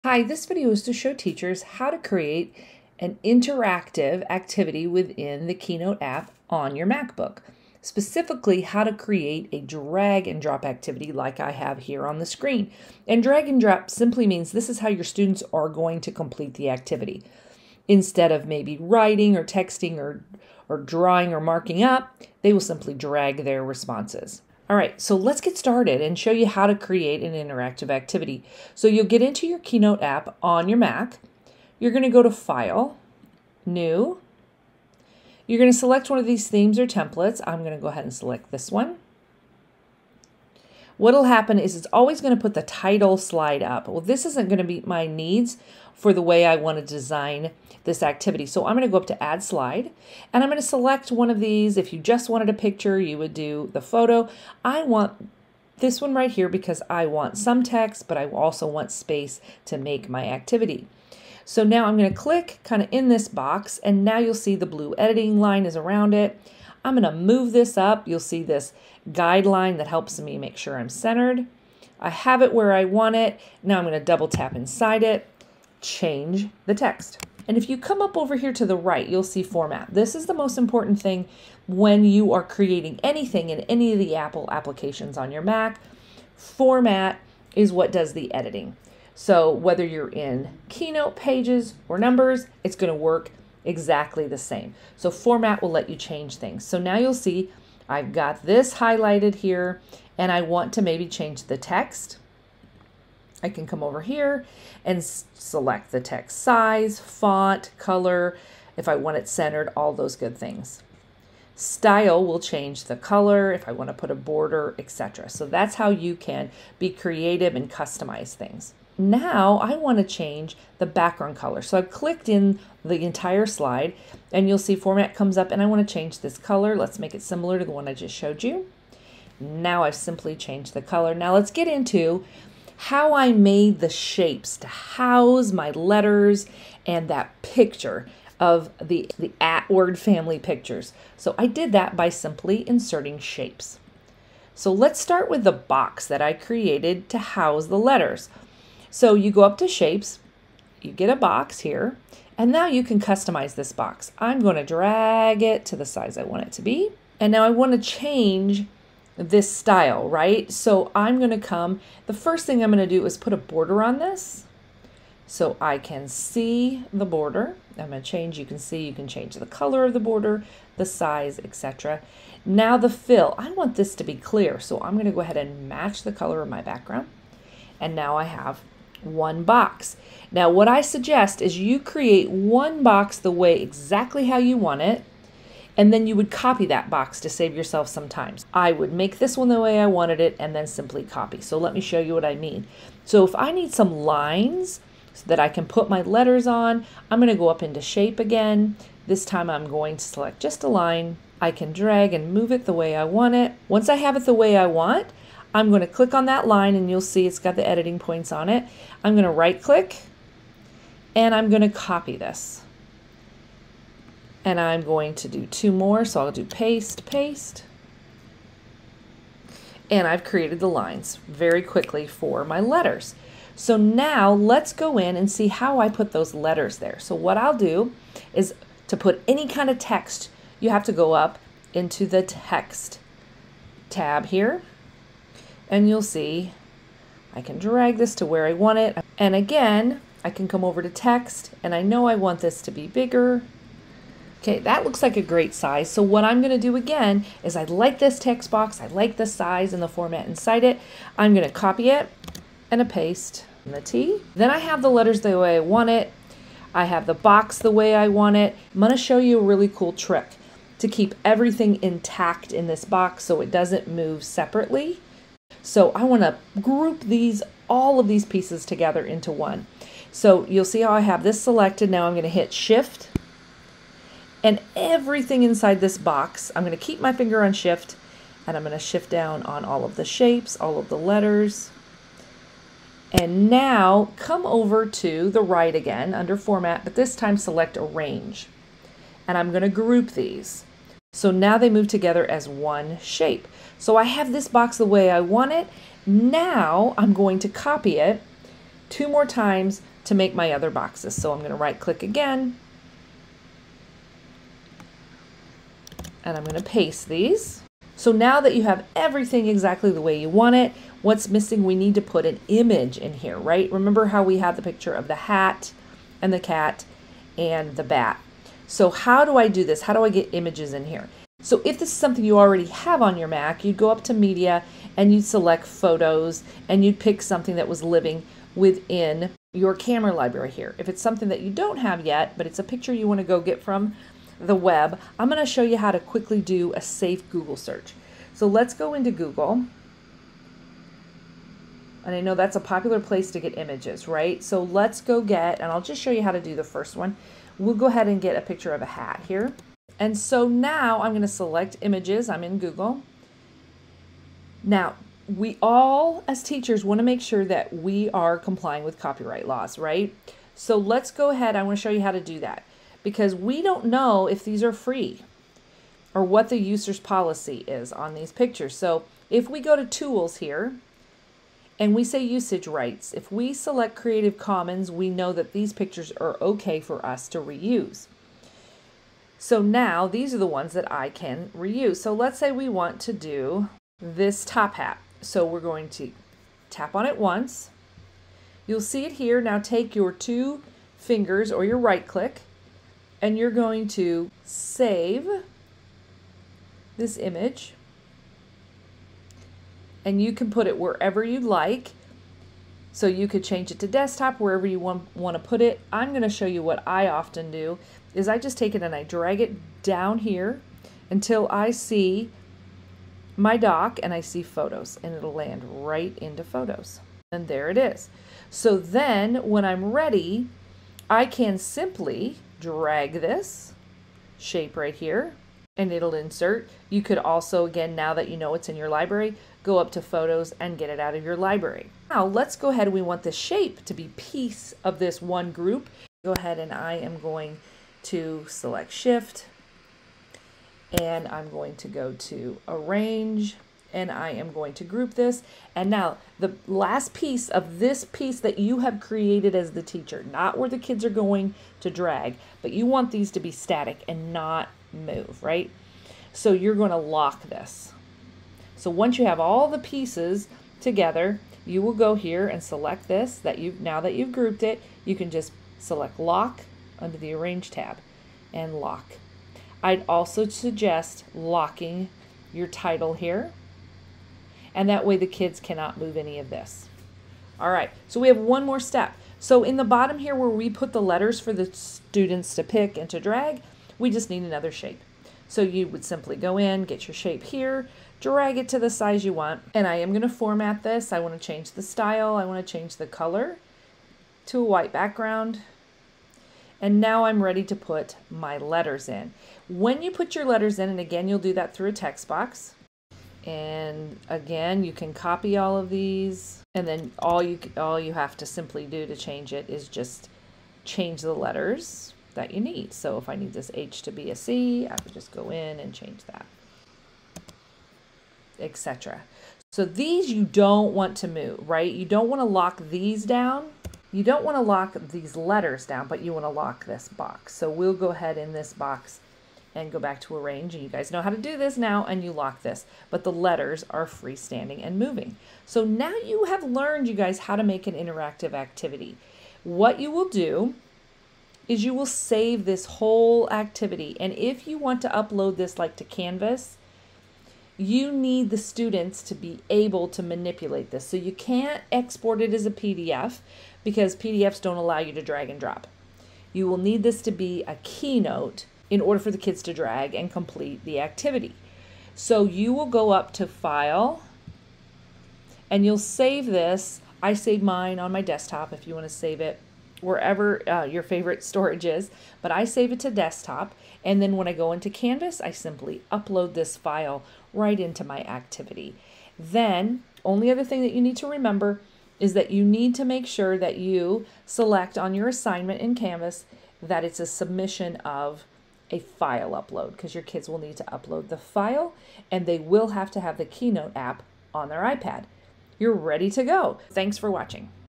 Keynote for Mac: Creating Drag & Drop Interactive Activities
This video is about Creating Drag and Drop Interactive Activities in Keynote